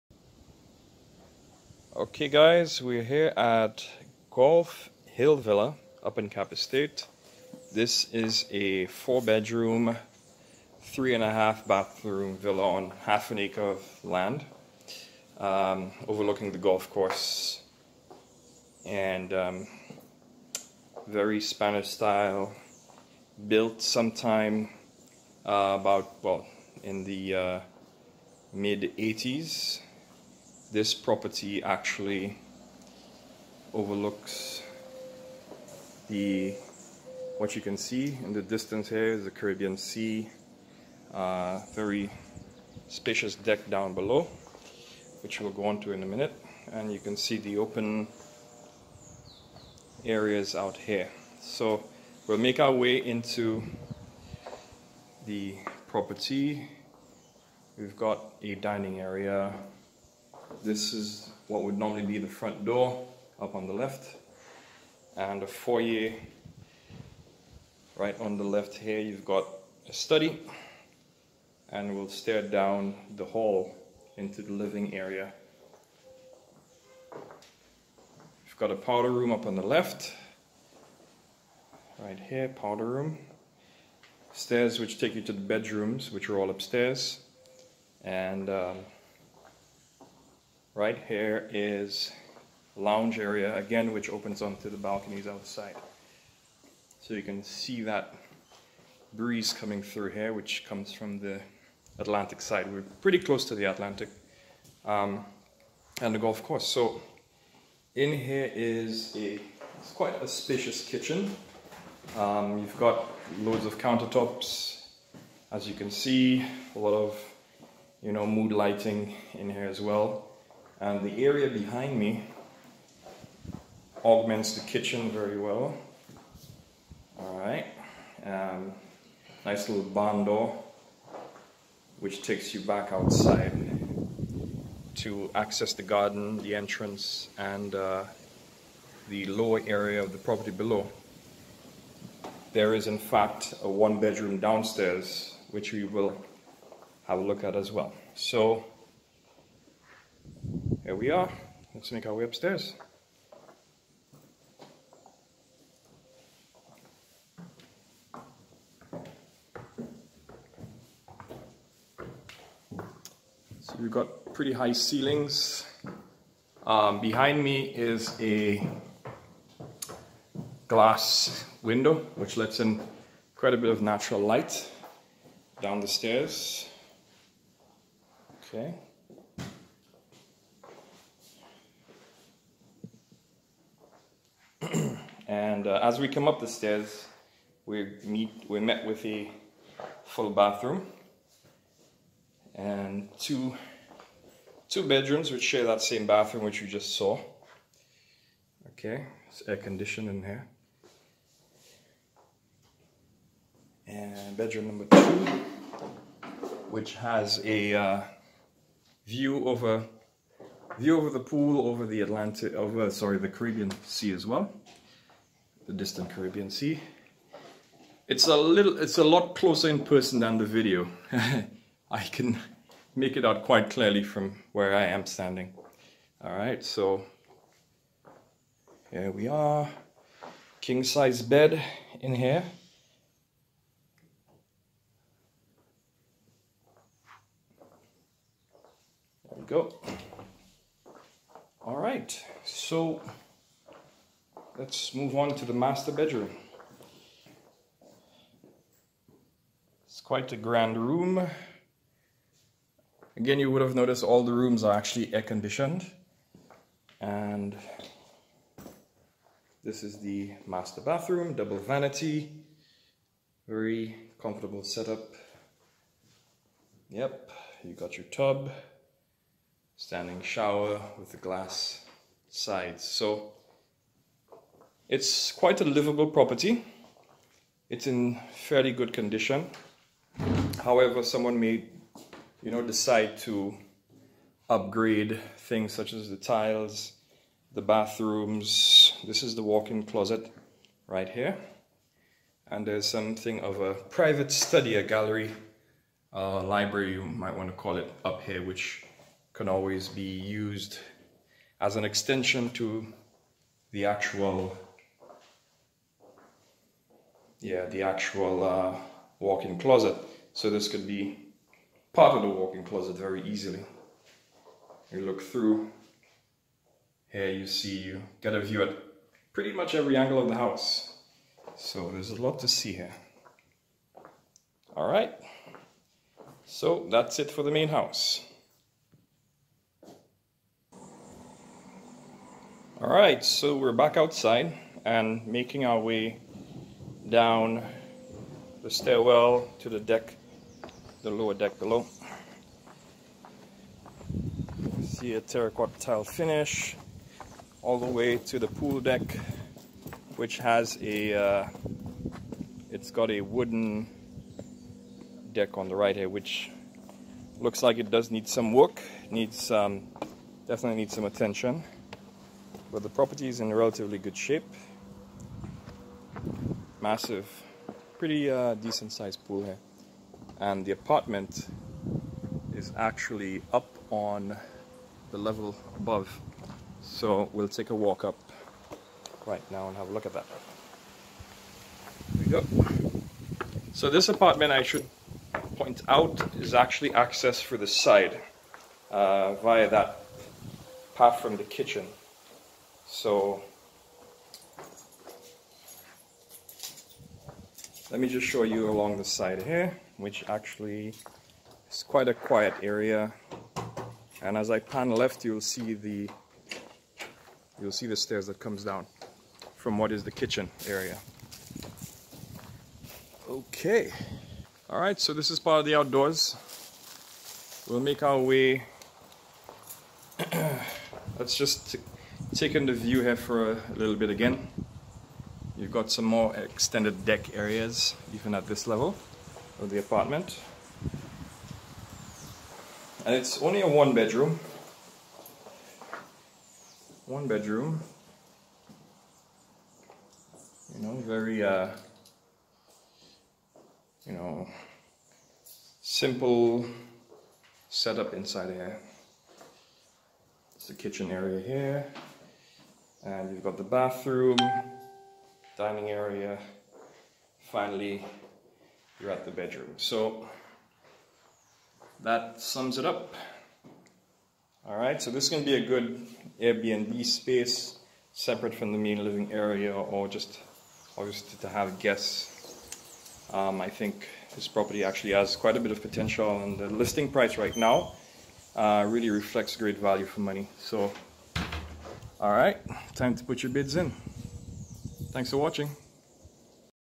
Golf Hill Villa Walkthrough - FOR SALE
Join us for a walkthough of this lovely villa overlooking the Cap Estate Golf Course